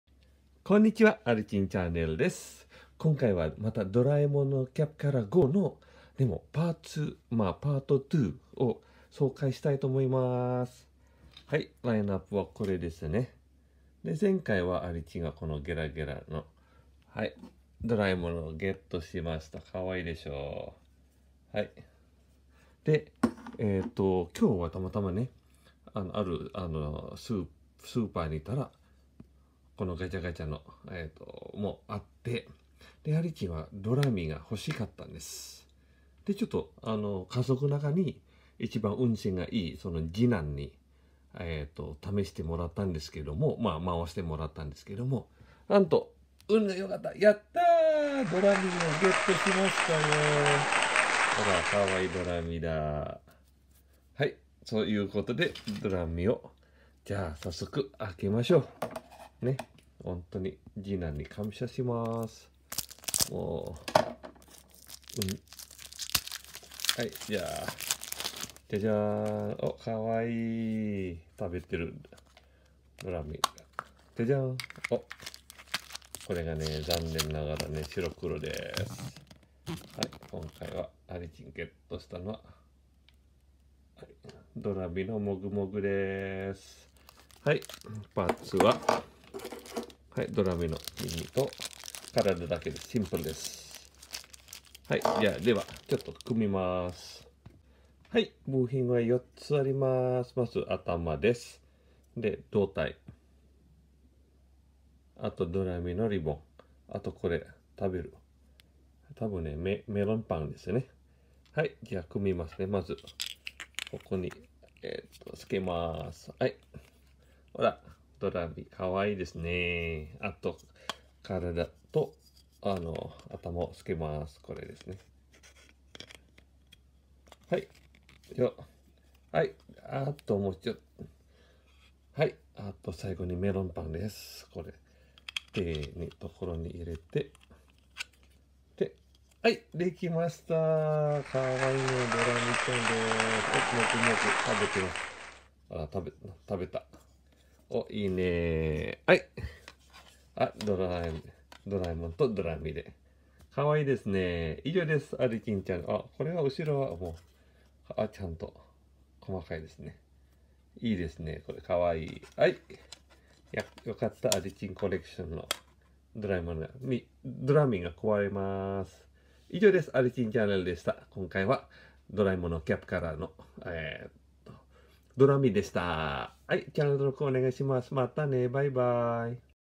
ドラえもんカプキャラ5のパート2 Doraemon Capchara 5 Part 2
今回、ドラえもんカプキャラ5のパーツ2を紹介します😉✨
パーツ1ではドラえもんをゲットしましたが、ドラミちゃんが欲しかったのでチャレンジしました😉
奇跡的にドラミちゃんをゲットしました😃